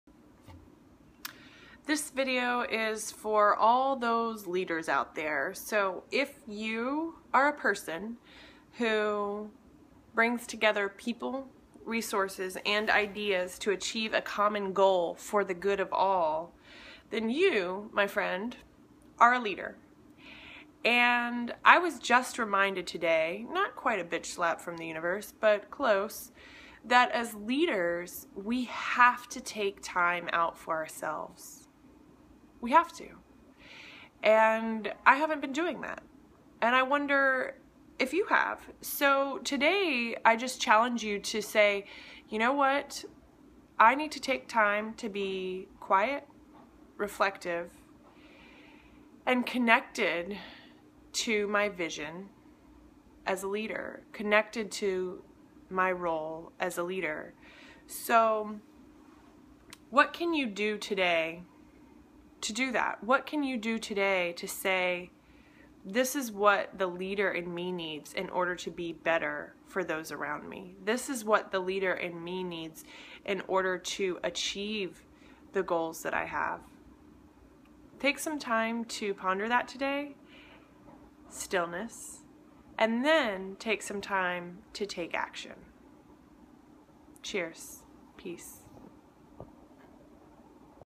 What is the number one thing you need to be a good leader?
Today I was reminded about something vital for all leaders- time for reflection and connection as well as time for action. What do you need to do today to be a better leader?

want more info on leadership, living a life on purpose, and business? sign up